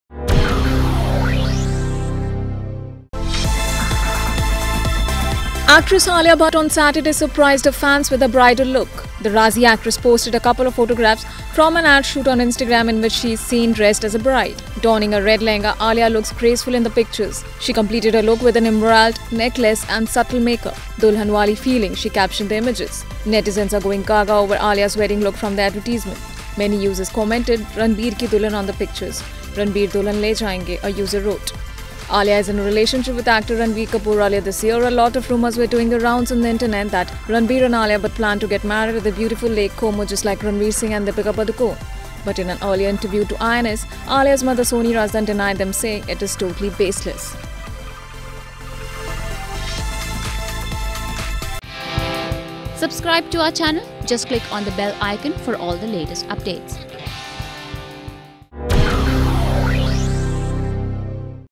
Alia Bhatt experiences her 'dulhan wali feeling'
Actress Alia Bhatt on Saturday surprised her fans with her bridal look.